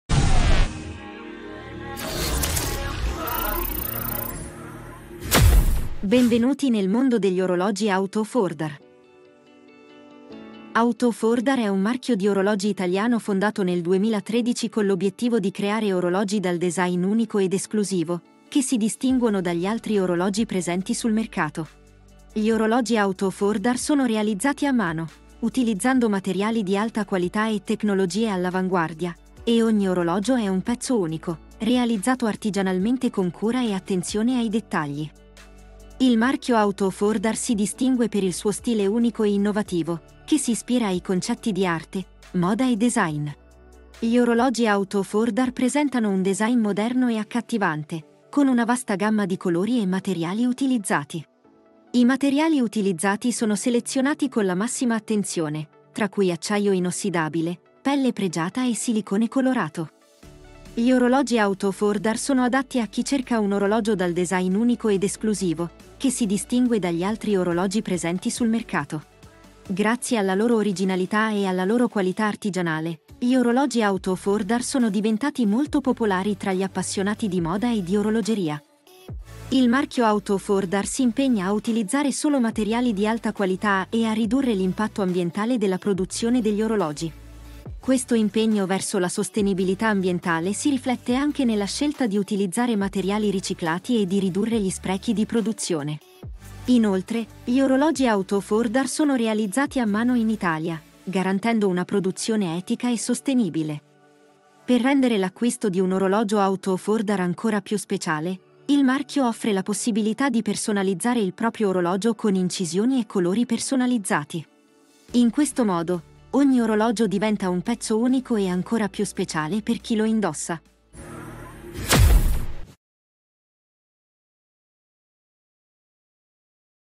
Out of Order: L'eleganza senza tempo degli orologi d'alta classe
Nel mondo frenetico di oggi, dove il tempo sembra sempre volare, gli orologi rappresentano un'ancora di eleganza e raffinatezza. Tra le prestigiose maison orologiere, spicca "Out of Order", un marchio rinomato per il suo design avanguardista e la precisione svizzera. In questo affascinante video, scoprirete come Out of Order catturi l'essenza di un tempo che sembra scorrere con lentezza, creando orologi che sono veri e propri capolavori da indossare al polso. Con sede nel cuore dell'orologeria svizzera, Out of Order è sinonimo di artigianalità e tradizione. I loro orologi, realizzati con materiali di altissima qualità e un'attenzione meticolosa ai dettagli, incarnano l'eccellenza che solo pochi marchi possono vantare. La combinazione di design contemporaneo e lavorazione artigianale tradizionale rende ogni pezzo unico, conferendo agli orologi Out of Order un fascino senza tempo. L'estetica degli orologi Out of Order è intrinsecamente legata alla filosofia del brand, che mira a rompere gli schemi e a sfidare i concetti tradizionali di design orologiero. I loro modelli si distinguono per la loro originalità, esplorando forme e colori unici che catturano lo sguardo di chiunque li incontri. Un Out of Order non è solo un orologio, ma un'affermazione di stile e personalità. Nel corso del video, potrete ammirare la bellezza e la versatilità di una collezione che comprende modelli per uomini e donne. Dagli orologi classici dal fascino intramontabile a quelli più audaci e contemporanei, Out of Order ha una varietà di opzioni per adattarsi a ogni gusto e occasione. Concludendo, se siete alla ricerca di un orologio che unisca design sofisticato e prestazioni impeccabili, la collezione di Out of Order è ciò che fa per voi. Guardate il video e lasciatevi conquistare dall'eleganza senza tempo di queste straordinarie creazioni orologiere.